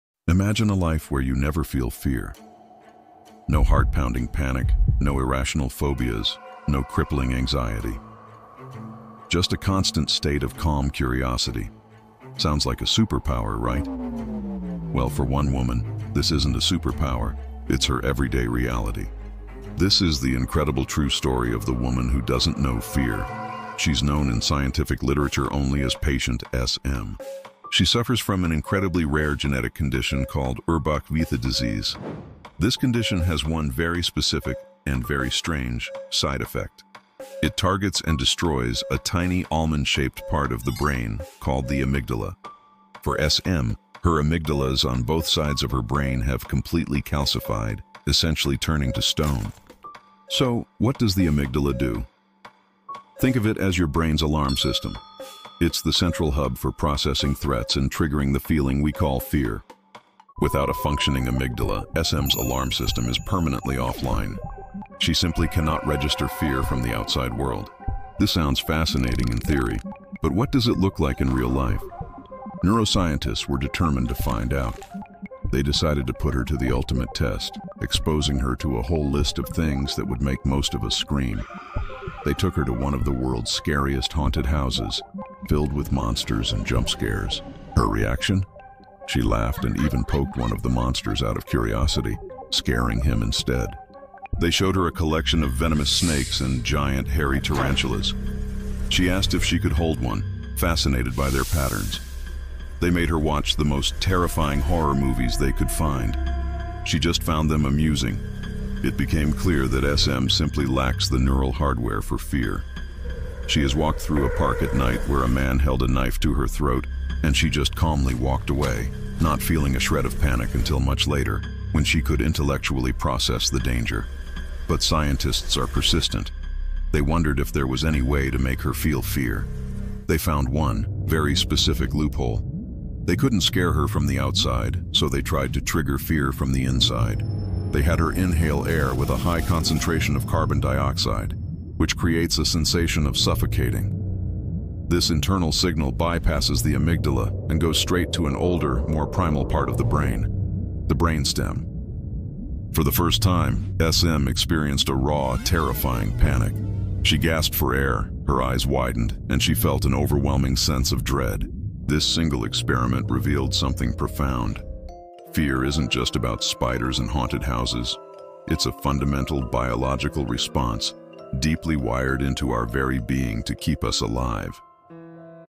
" The Woman with NO FEAR: Patient S.M. & The One Thing She Truly Fears !!
The Woman Who Can't Feel Fear | Patient S.M.'s Terrifying True Story

For decades, the American woman known only as Patient S.M. has been the subject of the most intense neuroscience research in history. Why? Because she truly cannot feel fear. This video dives deep into the detailed truth of her condition, the bilateral amygdala damage caused by Urbach-Wiethe disease, and the amazing experiments she endured.

Is her lack of fear a blessing or a curse? We reveal the one shocking experiment where she experienced absolute, overwhelming panic for the first time.